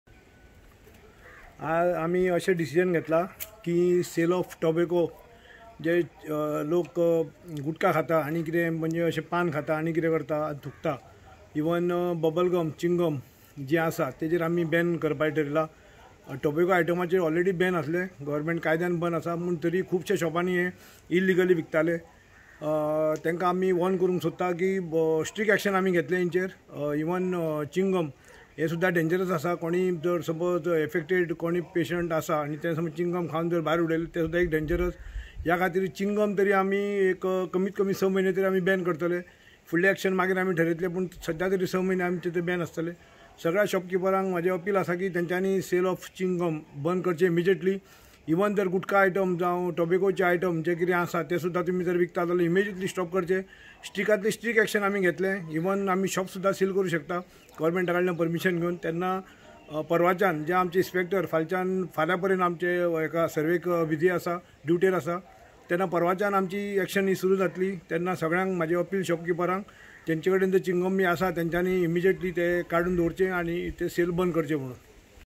CCP has decided to ban on Sale and Consumption of Chewing Tobacco says Panaji Mayor Uday Madkaikar
CCP has decided to ban on Sale and Consumption of Chewing Tobacco in all forms.Also ban on Sale of Chewing gum.